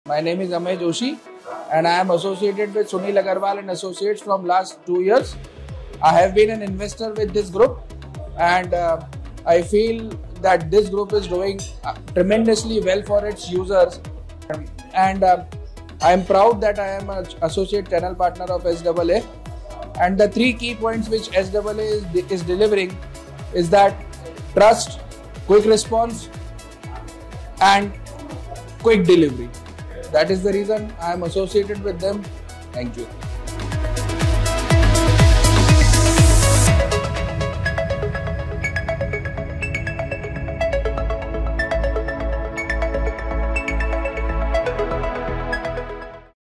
Testimonial of Our Channel Partner Mr Amay - Sunil Agrawal and Associates
Team SAA believes in the concept of growing together and recognizing the good work put forward.
Celebrating our channel partners and awarding them for their diligent work every day.
Congratulations to all the awardees. We look forward to more such future successes together.